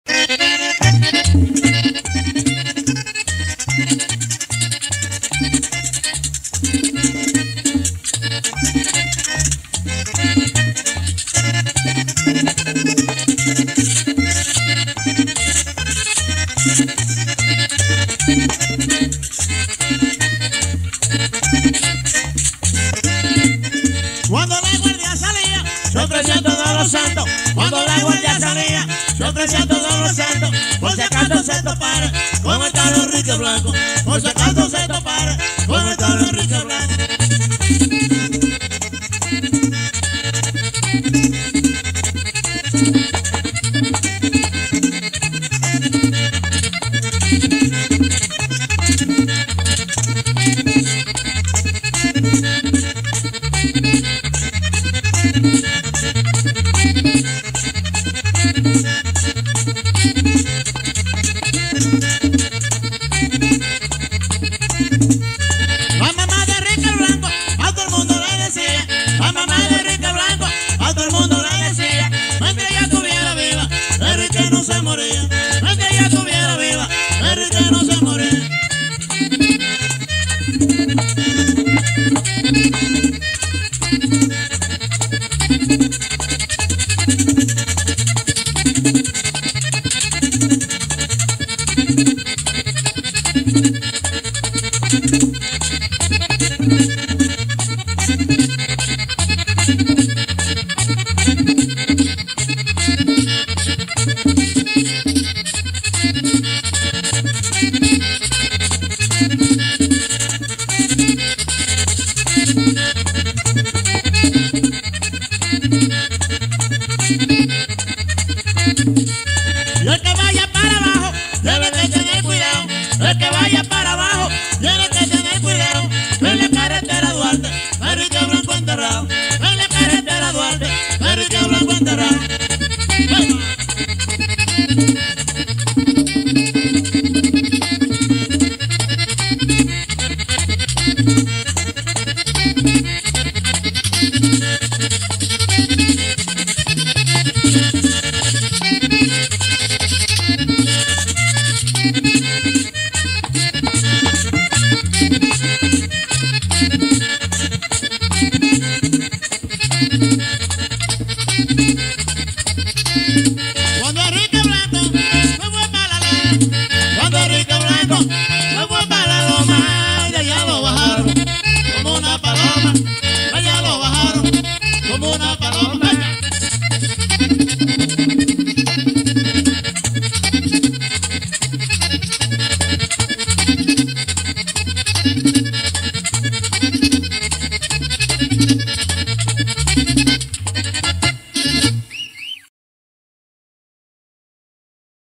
Siano Arias Henríque Blanco - A Cuarteto 1990.
Siano Arias junto A: Ramoncito güira, Boca Chula Tambora y Alejandro Bass.